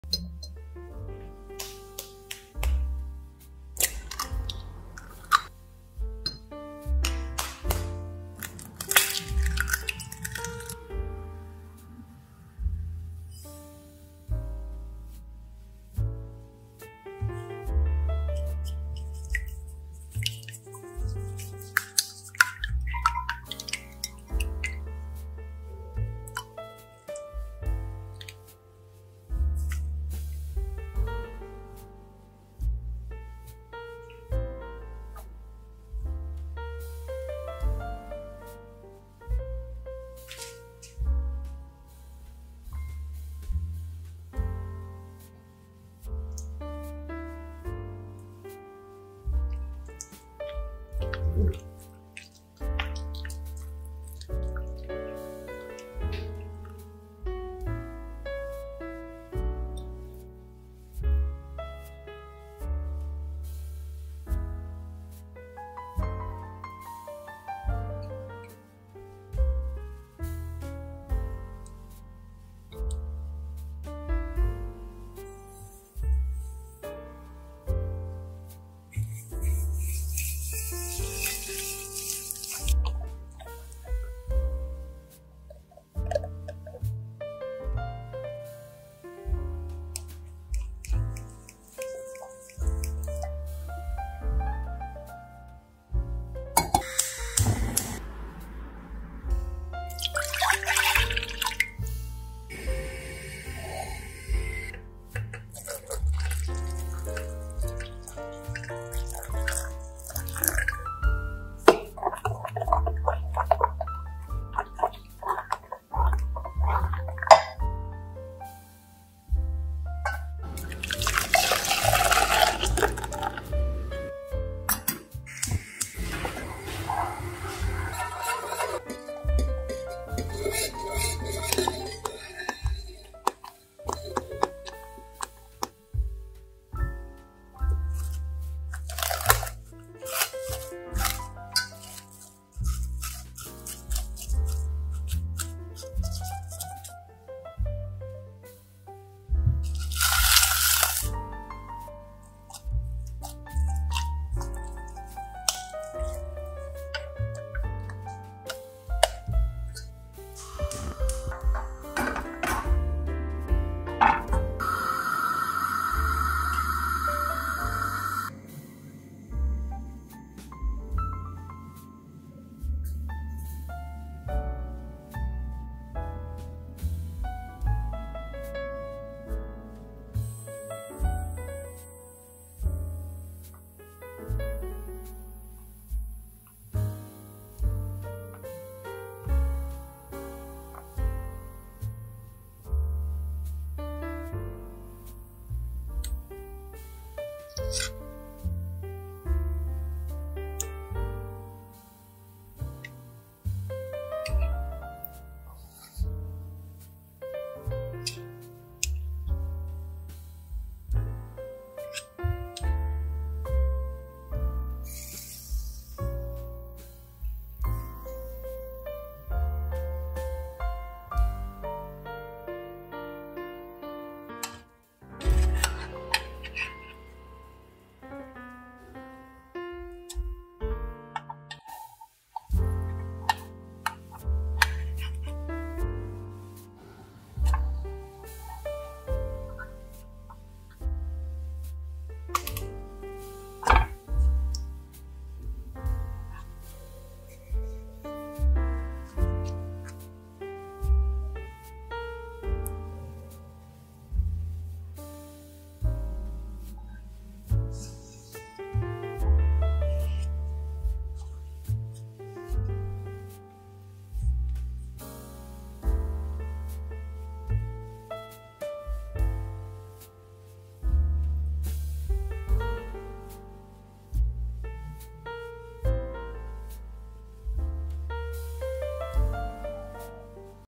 No Oven - No Machine - How to Make Custard Cream Pancake
hello guys we made so incredibile looks and delicious Custard Cream Pancake i think easy and good for breakfast or lunch and i think your home guess love this recipe ENJOY

▶Dough
180g soft flour (cake flour)
2eggs
200g milk
30g sugar
2g salt
30g cooking oil
3g baking powder

▶ Custard cream
3 yolks
300g milk
60g sugar
20g corn starch